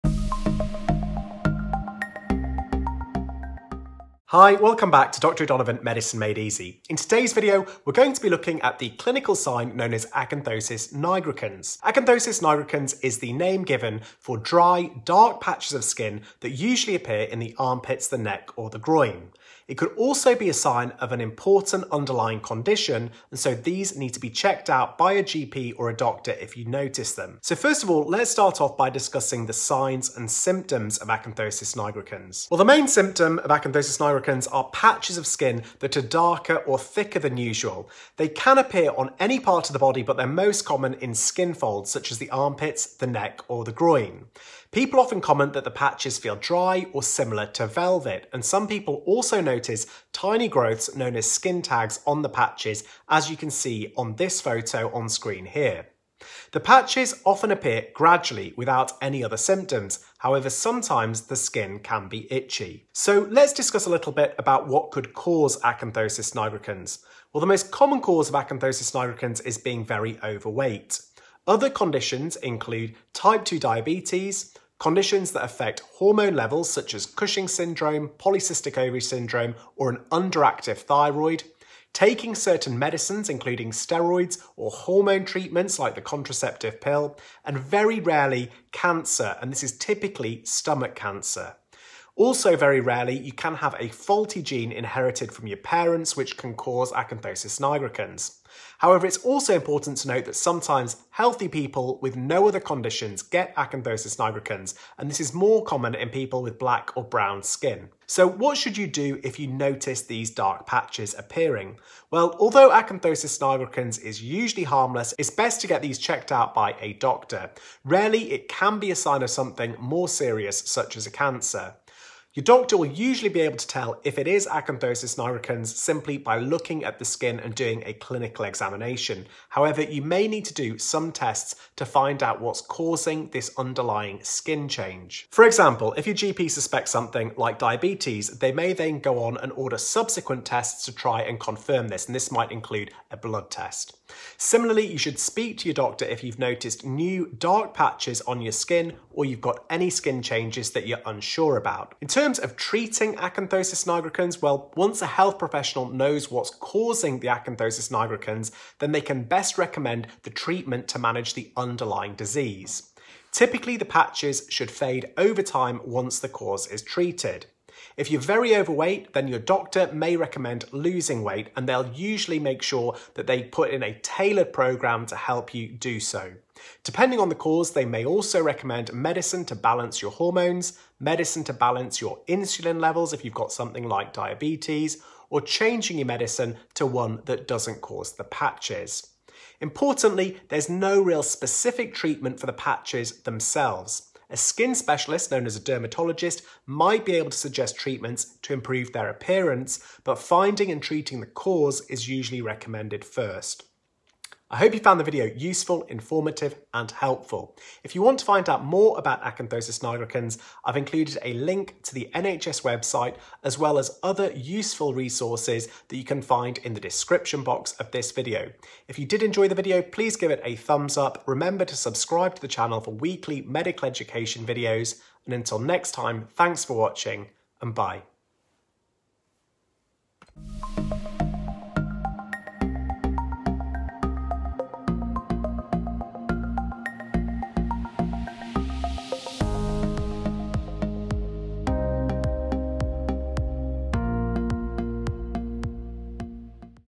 Doctor explains Acanthosis Nigricans - signs, symptoms, causes, treatment and more!
In todays video we are going to take a look at the clinical sign Acanthosis nigricans. Acanthosis nigricans is the name for dry, dark patches of skin that usually appear in the armpits, neck or groin. It could be a sign of an underlying condition, so it needs to be checked by a doctor.

Possible causes include:

• Obesity , Type 2 diabetes
• conditions that affect hormone levels – such as Cushing's syndrome, polycystic ovary syndrome or an underactive thyroid
• taking certain medicines – including steroids or hormone treatments like the contraceptive pill
• rarely, cancer – usually stomach cancer
• rarely, a faulty gene inherited from your parents
Sometimes healthy people with no other conditions get acanthosis nigricans. This is more common in people with black or brown skin.